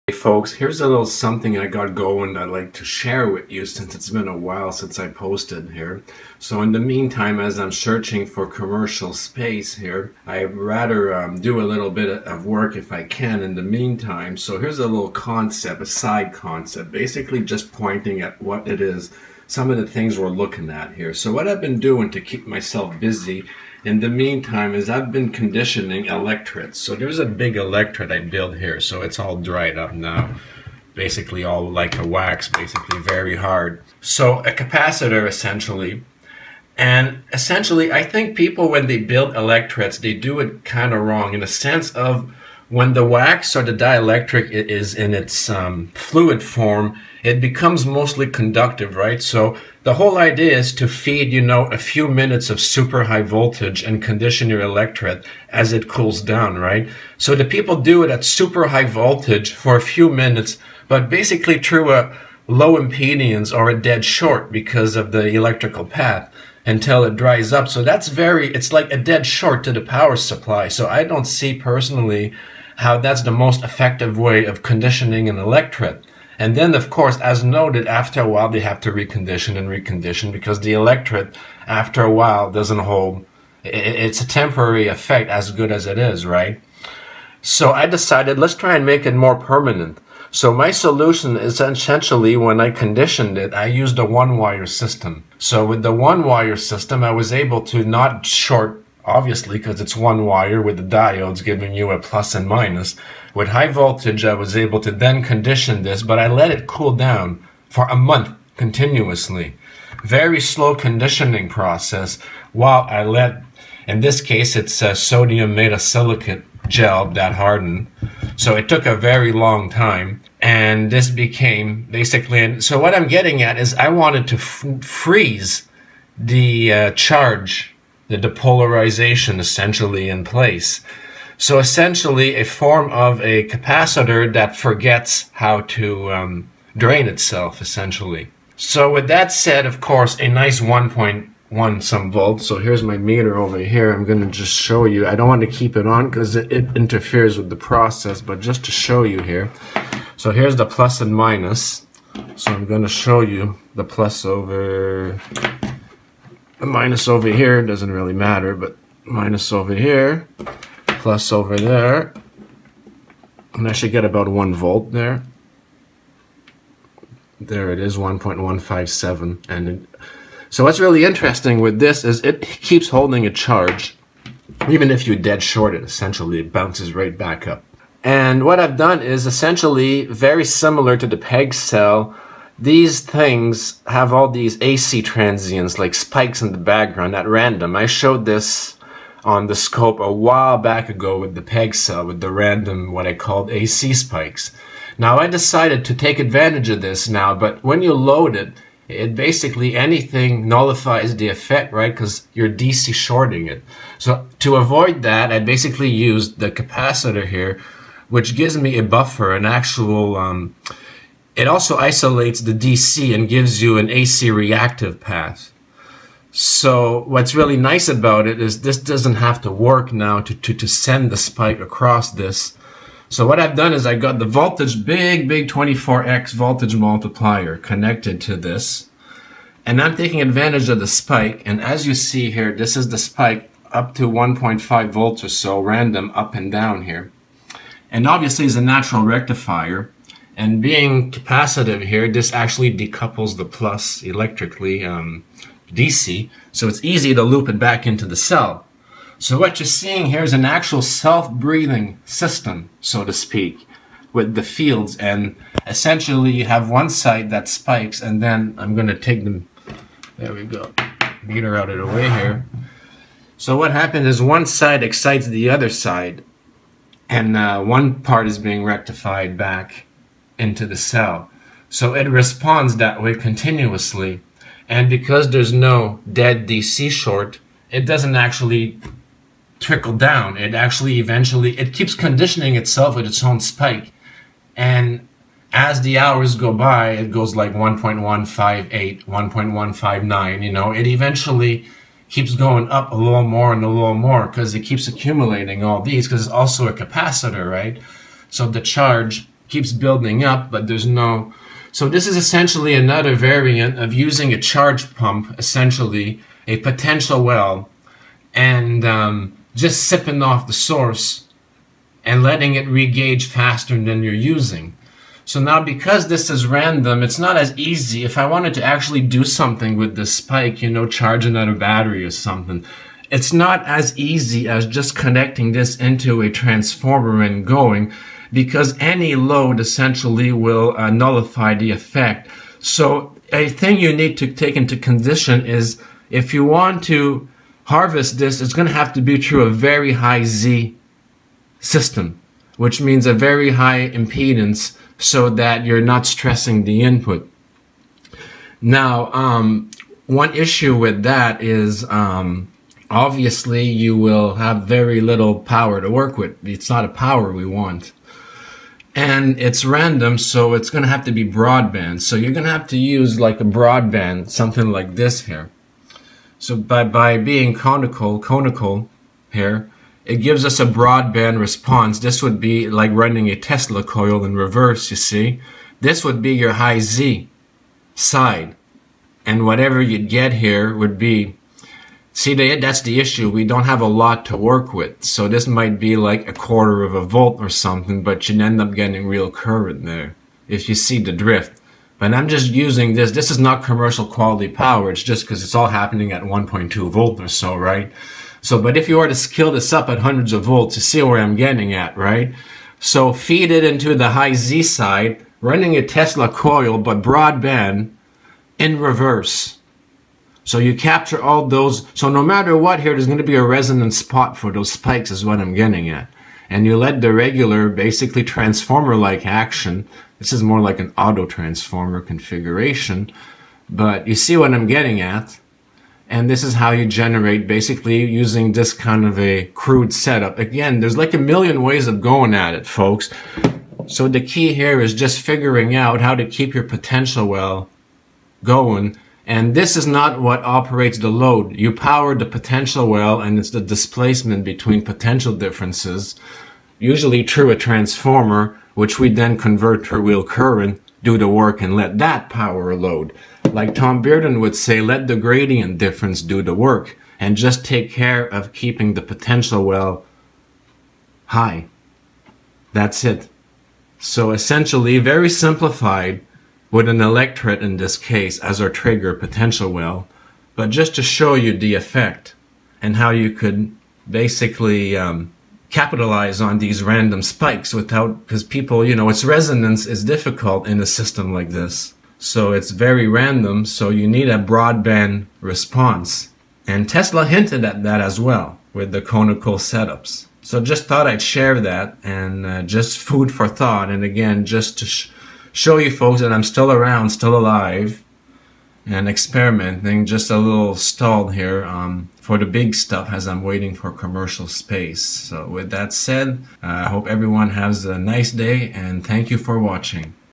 Living Electret
Good day, folks. Here's a demonstration of what I’d call a live, self-regulating, “breathing” electret capacitor—a dynamic potential well in action. I’ll also share some thoughts on how to sustain the energy loop and maximize the output spike from such a system.

This particular electret took about a month to fully condition under high voltage using a one-wire input—essentially a trigger method—allowing it to gradually settle and “harden” into a stable polarized state. The polarity has now locked in without placing stress on the input power supply, which often happens when trying to force a closed-loop condition during the soft, conducting phase of the dielectric. That stage typically acts like a large resistor, or even a near-dead short, drawing heavy current during traditional conditioning.

My goal was to sidestep that inefficiency—to illuminate a smarter approach to conditioning that respects the dielectric's evolving state and avoids unnecessary drain on the supply. This method may offer insights into creating a more energy-conscious electret system, potentially useful in open-loop or field-coupled configurations.